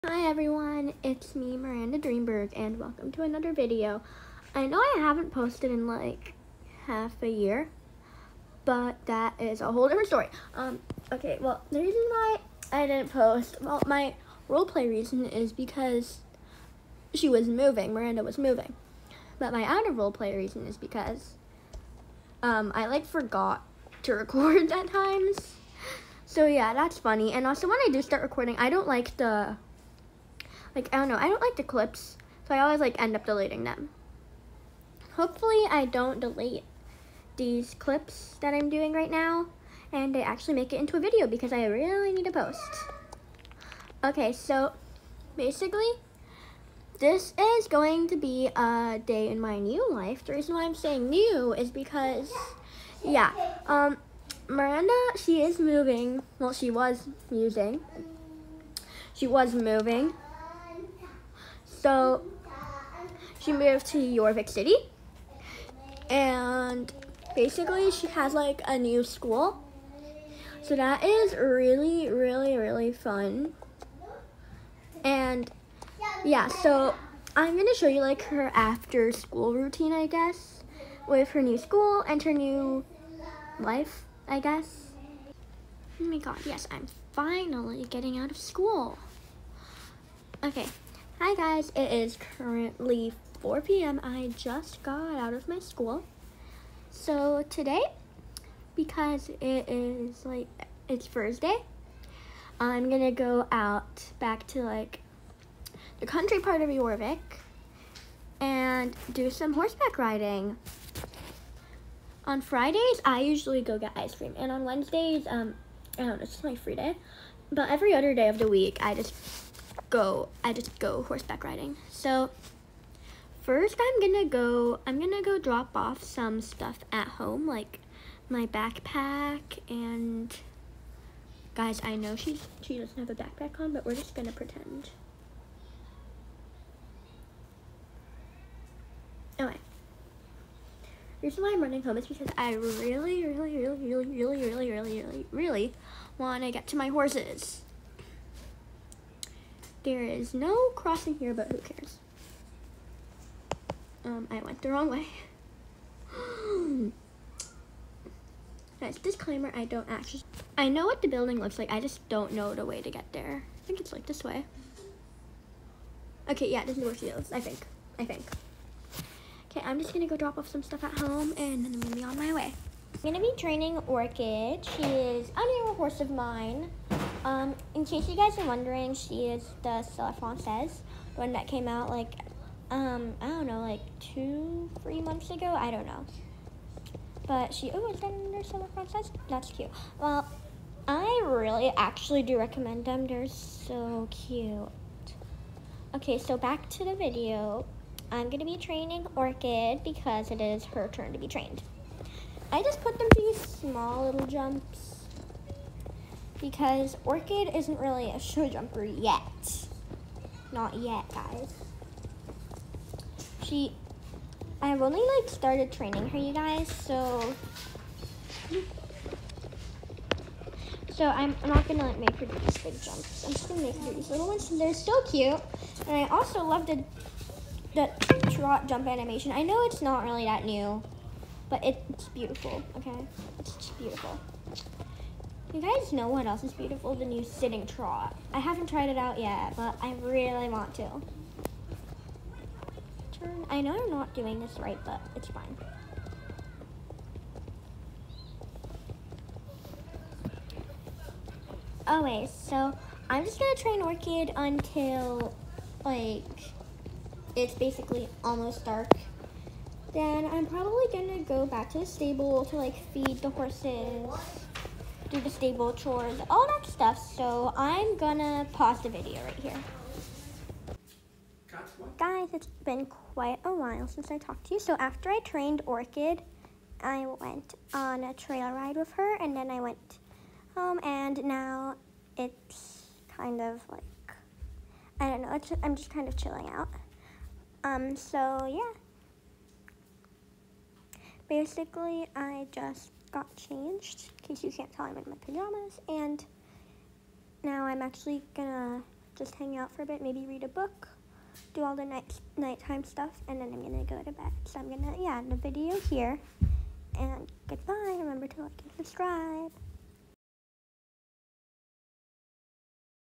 My SSO day routine
Heyyyyyyy i know its been forever since i last posted but im here with a new video! And yes, miranda has moved and she is at a new school! Enjoy :)